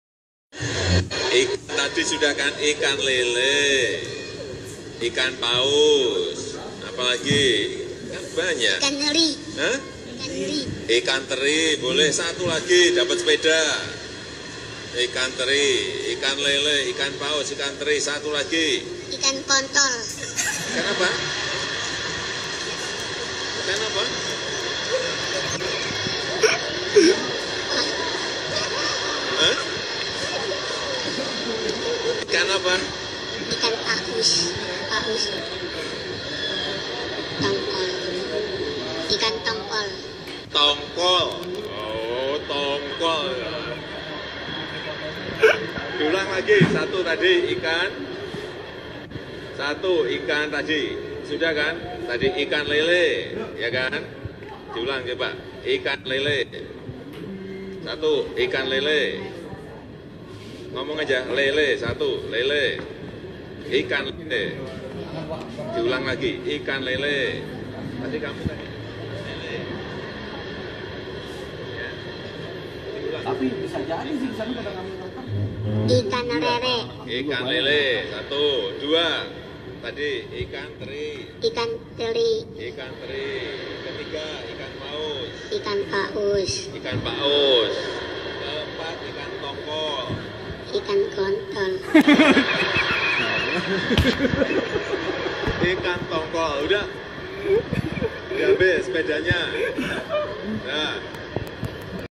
Salinan dari Bocah ini ngomong "KON*OL" di depan presiden Jokowi
Ikan kontol di depan presiden jokowi